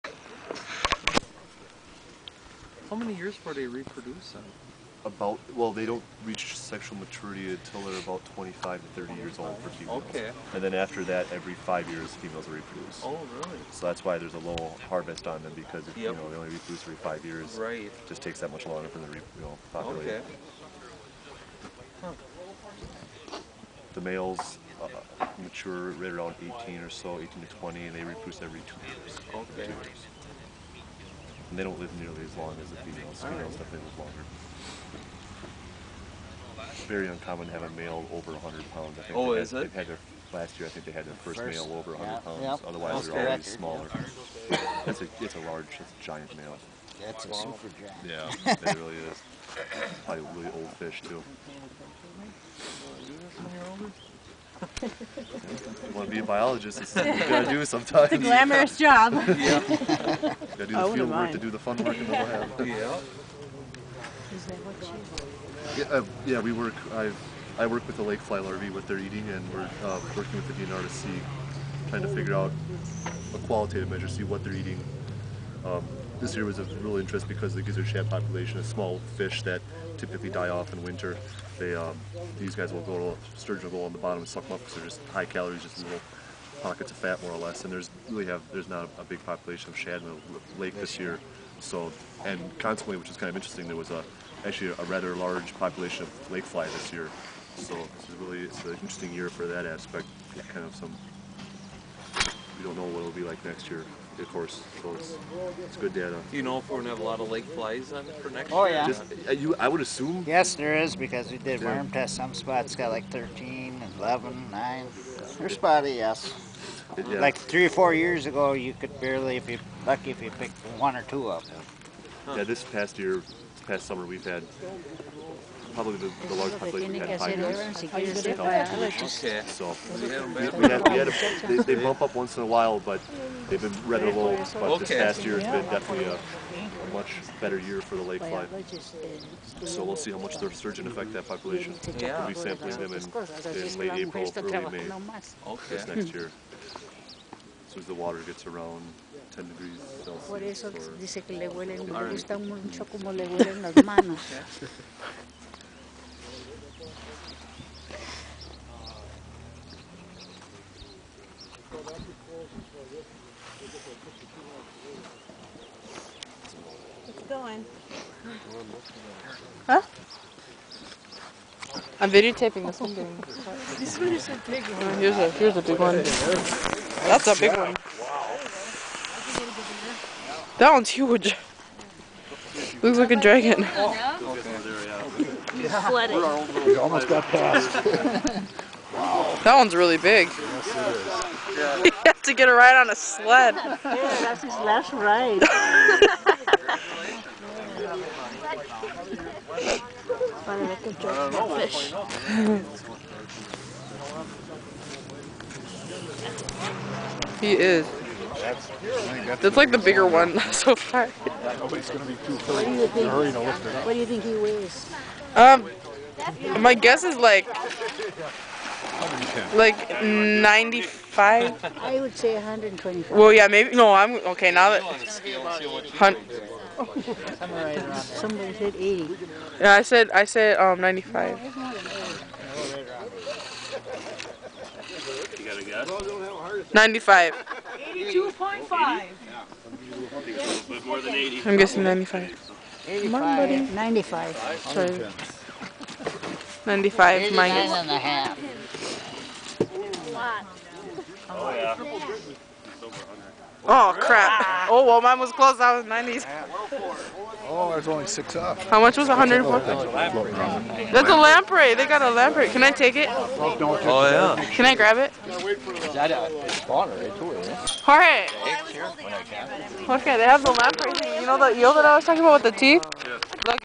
Cutting open a sturgeon
a biologist cutting open a sturgeon. at the end i found a lamprey eel